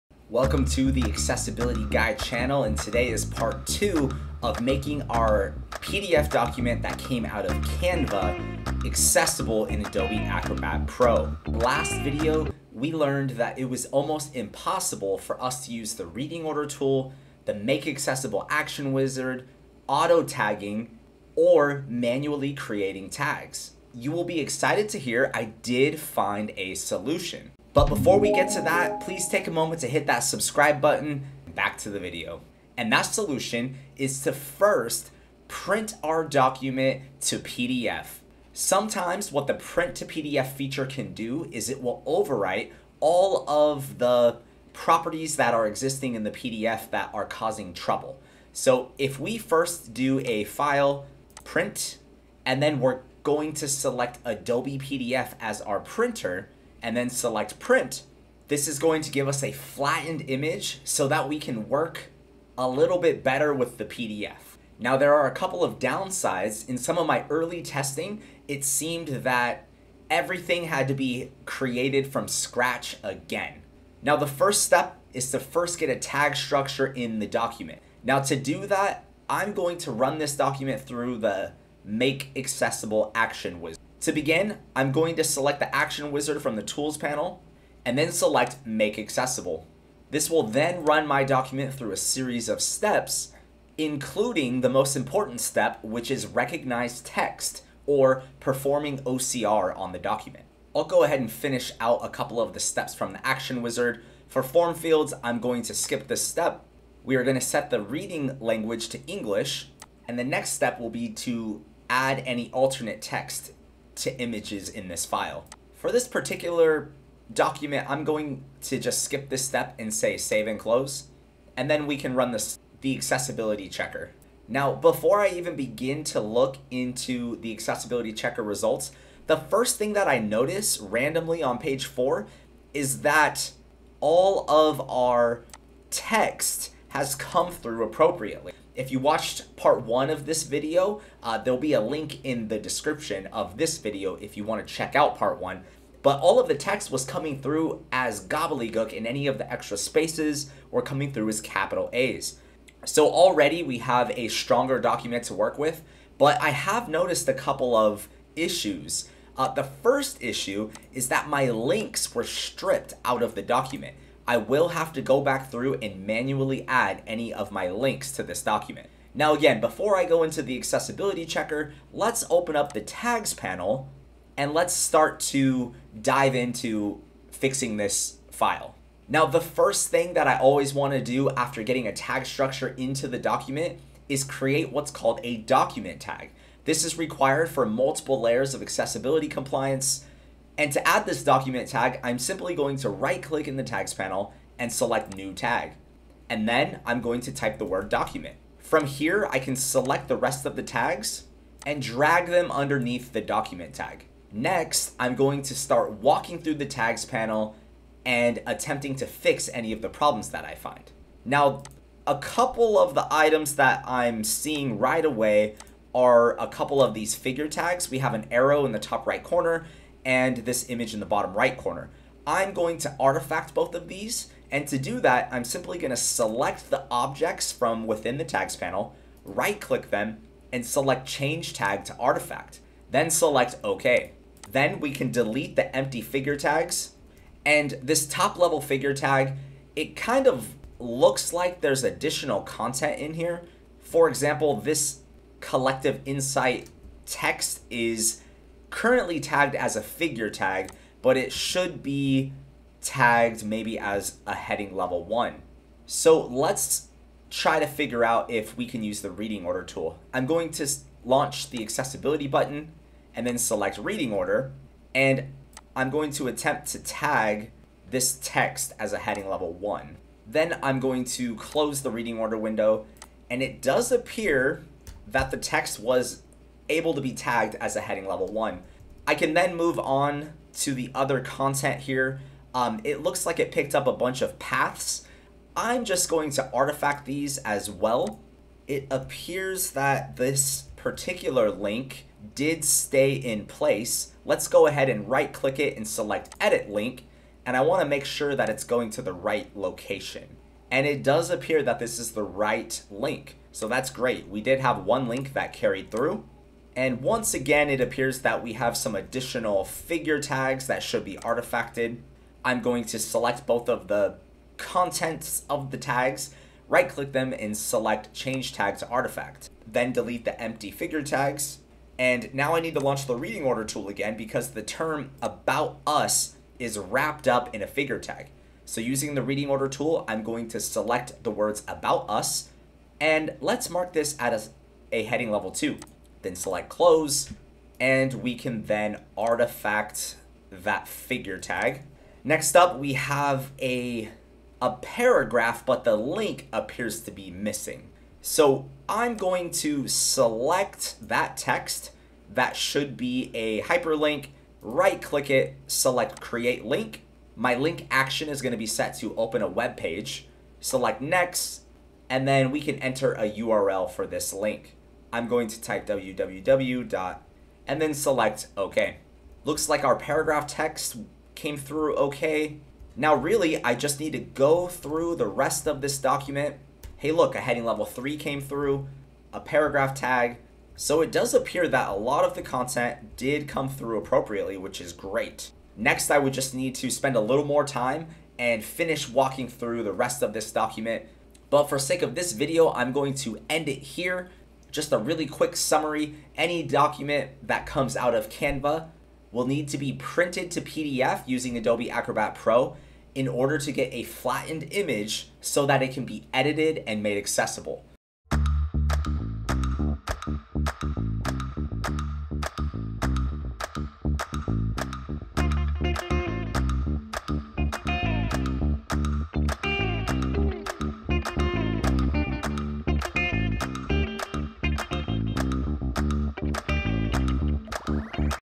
Canva PDFs - the Solution | Part 2
The solution to working with Canva PDFs is to use the Print to PDF function.

0:25 - The Solution
1:16 - Get a tag structure into the document
3:08 - Create a document tag
3:25 - Move tags under the document tag
3:46 - Artifact content
4:21 - Use the Reading Order Tool
5:51 - adding a link
6:33 - Summary

Canva graphics are not accessible. Start using Venngage today